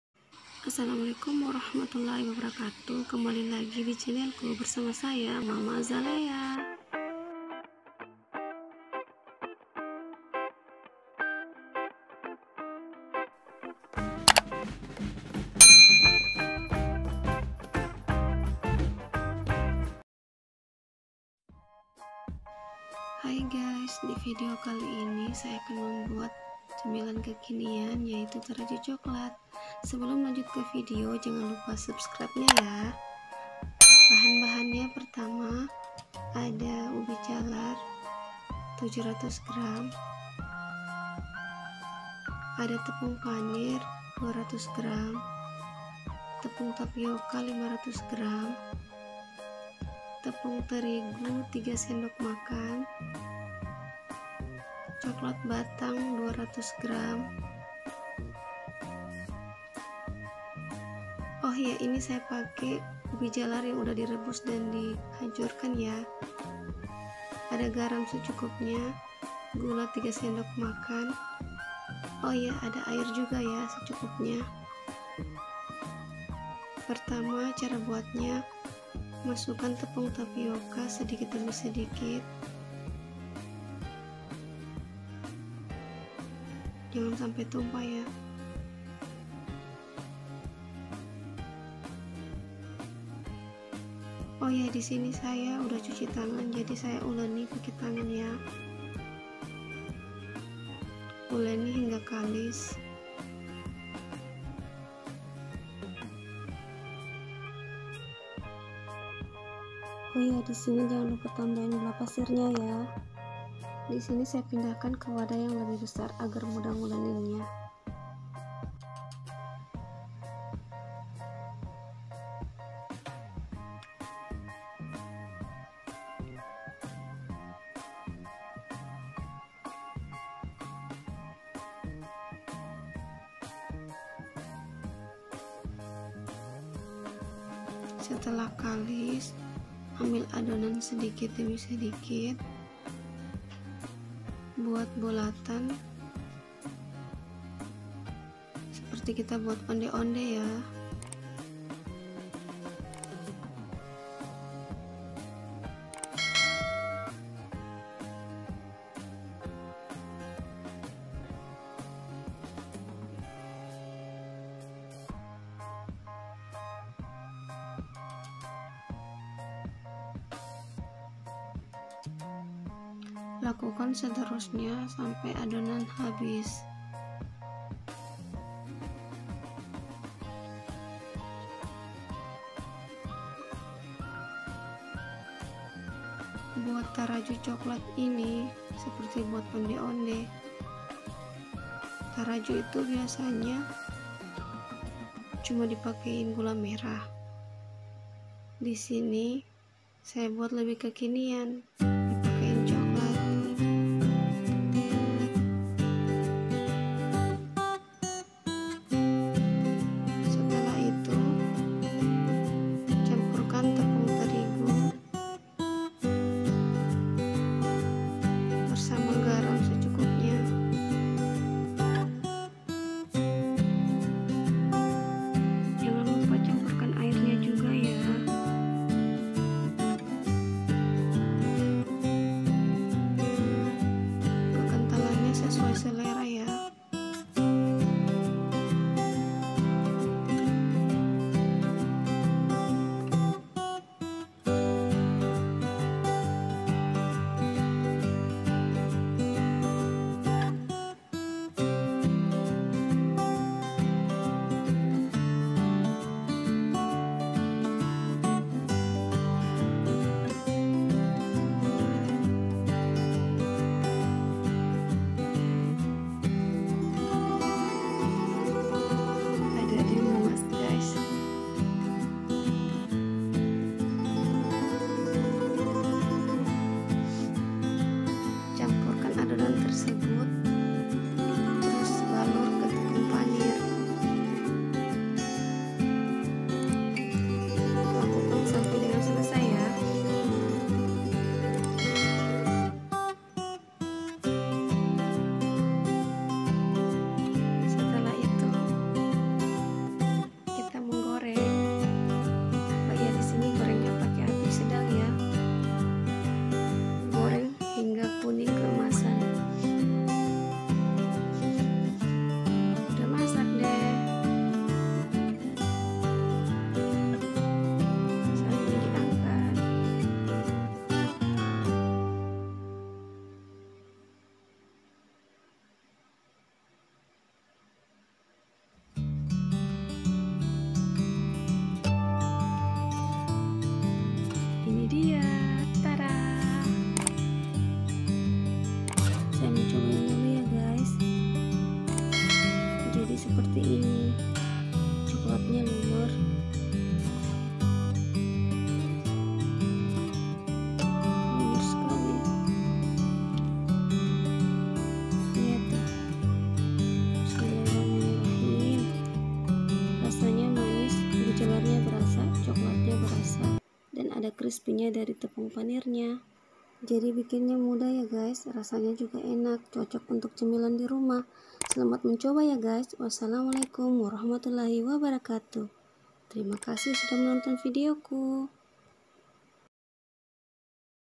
Gampang!! cara membuat kue tarajju ubi jalar lumer|tarajju khas bugis
Taraju  makanan khas Sulawesi
TARAJU coklat
       Bahan-bahan membuat tarajjunya
-Ubi jalar 700gr
-TepungbTapioka 500gr
-Coklat batang 200gr
-Tepung terigu 3sendok makan
-Tepung panir 200gr
-Gula 3 sendok makan
-Garam secukupnya
-air secukupnya
         Cara membuat Tarajju
-Campurkan ubi jalar,tepung tapioka dan gula pasir uleni hingga Kalis,,setelah Kalis ambil sedikit adonan buat bulatan masukan coklat batang tutup kembali (seperti buat onde-onde)
Setelah selesai,,,,
-Diwadah lain campurkan tepung garam n air,,kekentalan sesuai selera kemudian  masukan adonanx n Balur ke tepung panir setelah itu goreng hingga kuning ke emasan

bola ubi coklat lumer, bola bola ubi coklat lumer, bola bola ubi isi coklat